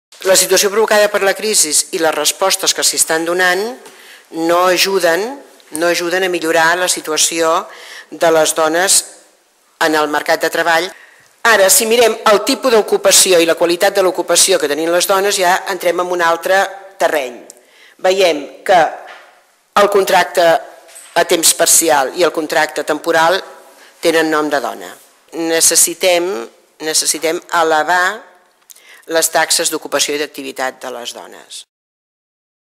Dones i mercat de treball
Declaracions de Rosa Bofill, Secretària de la Dona de CCOO de Catalunya, en roda de premsa per presentar l'informe sobre la situació de les dones en el mercat de treball (4-3-2011).